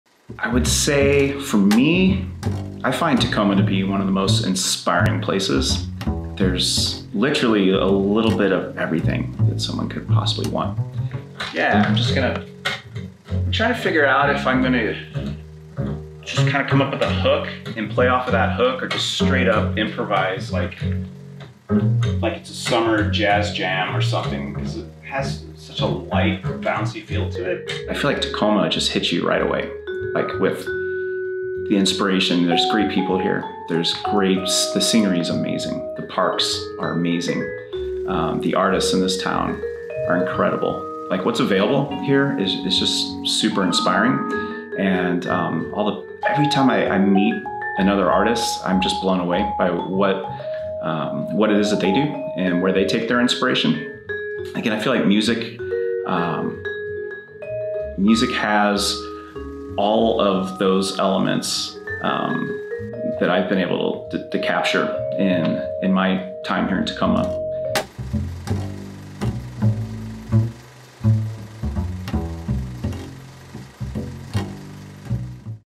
Ten of Us Musician Highlight | James Doyle
“What’s available here is inspiring.”

Percussionist @jwdoylepercussion shares with us his love for Tacoma and the influential parallels he sees between his art and his city.

tenofus | 2023
powered by @fosterscreative x @etctacoma